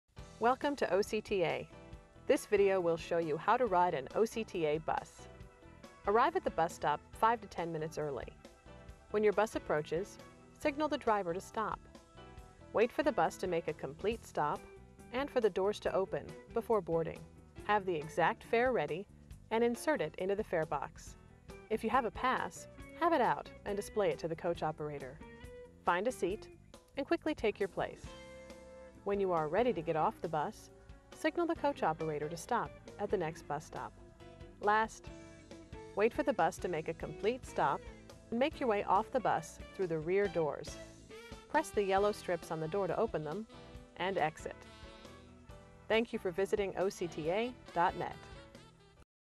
How to Ride a Bus with Bike
For nearly 10 years, HalfCity Productions (now known at Deb Squared Photo) produced video and still photography for the Orange County Transportation Authority (OCTA) . This video shows how to ride an OCTA bus with a bicycle